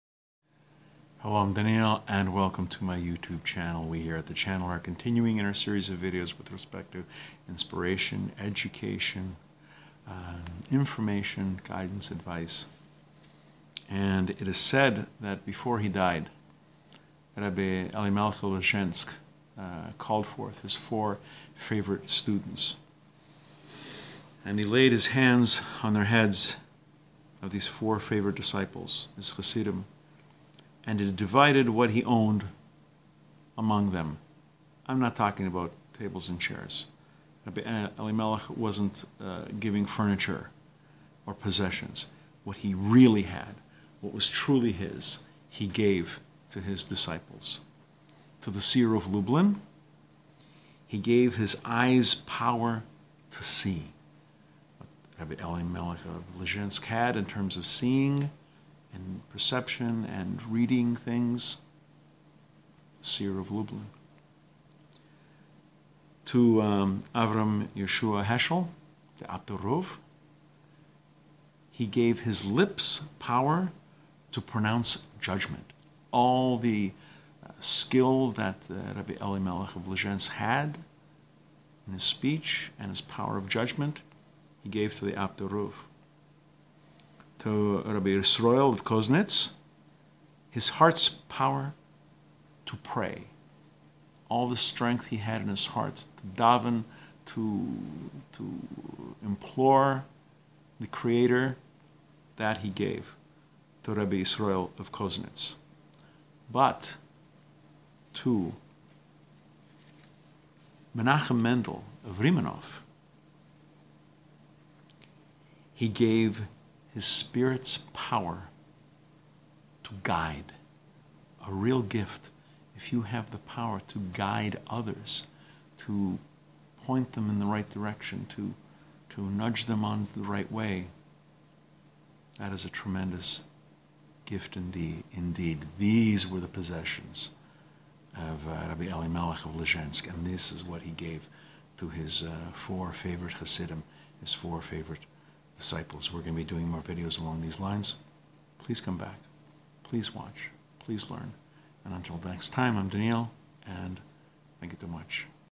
R' Elimelech's Four Bequests [My 700th video]
(700)
R' Elimelech of Lizhensk. The Seer of Lublin. The Apter Rebbe. R' Israel of Koznitz. R' Menachem  Mendel of Rymanov.